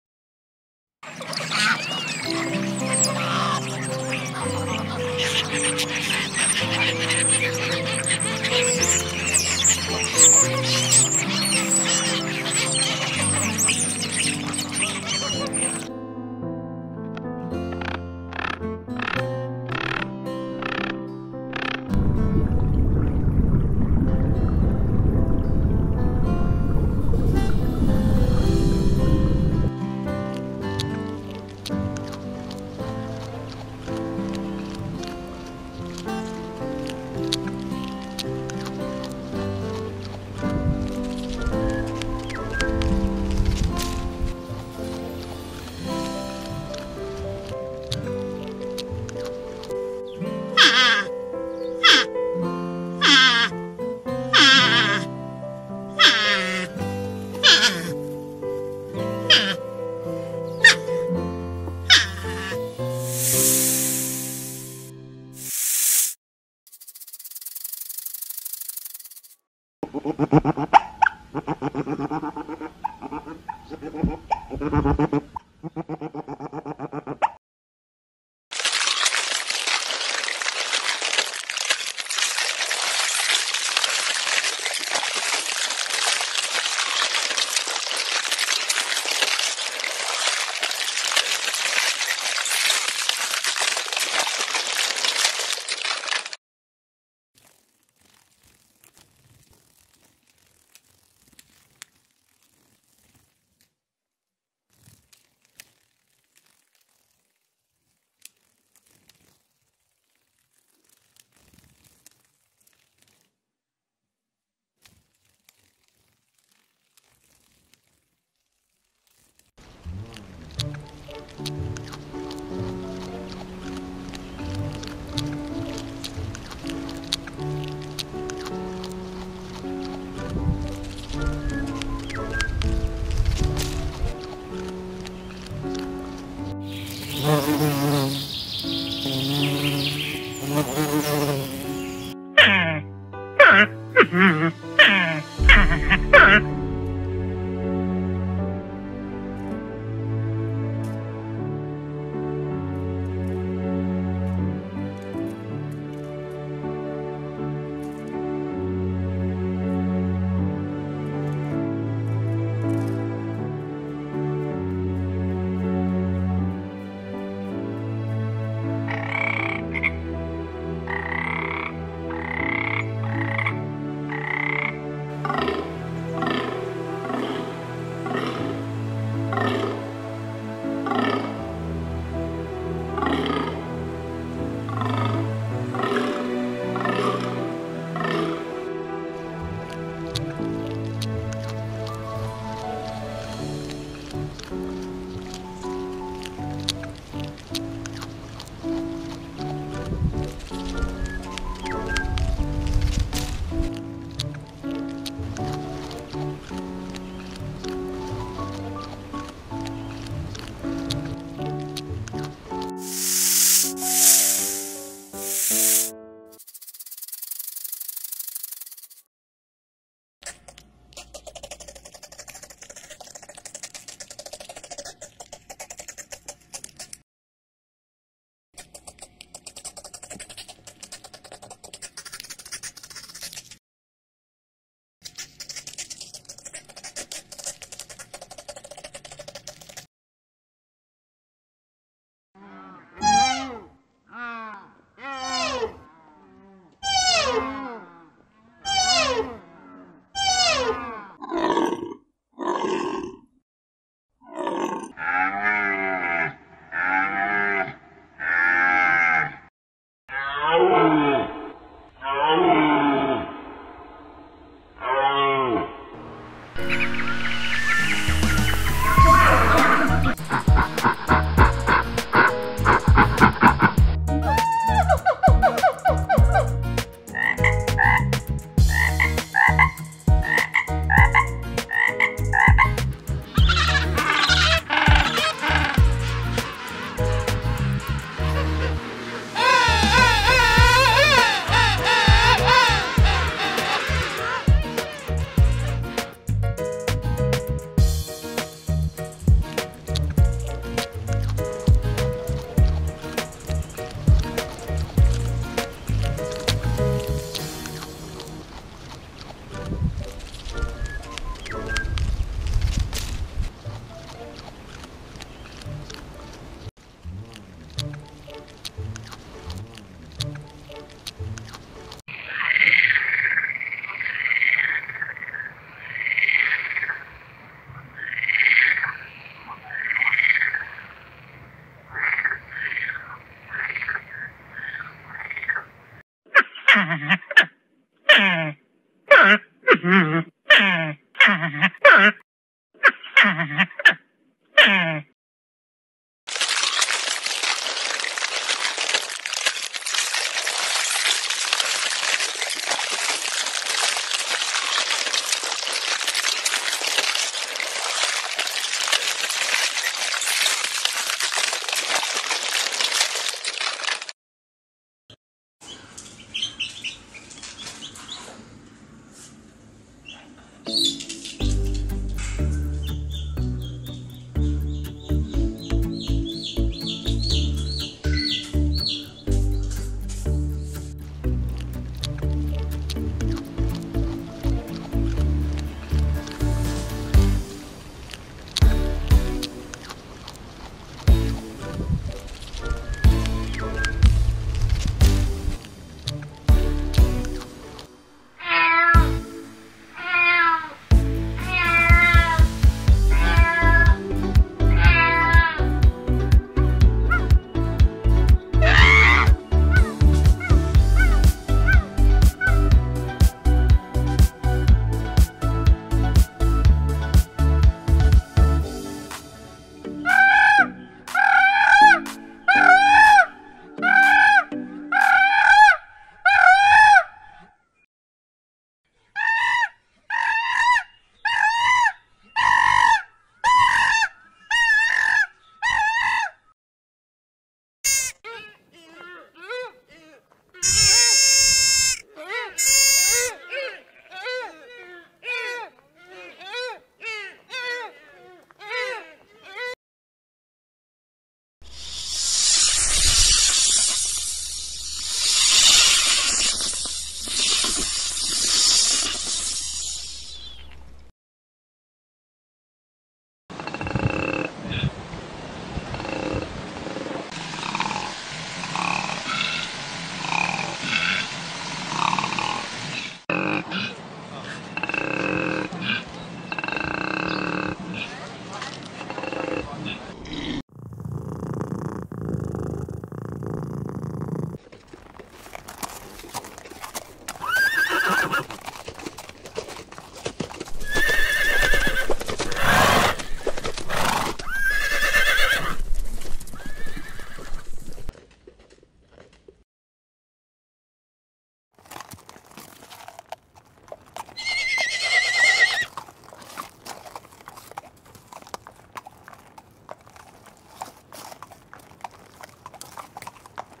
The lives of familiar animals: Hippopotamus, Bird, Giraffe, Polar bear, Moose, Zebra, horses, ducks
🐯Cute moments of pets: Dog, Cow, Cat, Bee, Pig, Chicken, Elephant,...
Funny and adorable moments of animals around us. The chicks are looking for their mother, the cats are cooing with their owners, the dog is barking, the giraffes are eating, And many other interesting moments in the lives of the animals.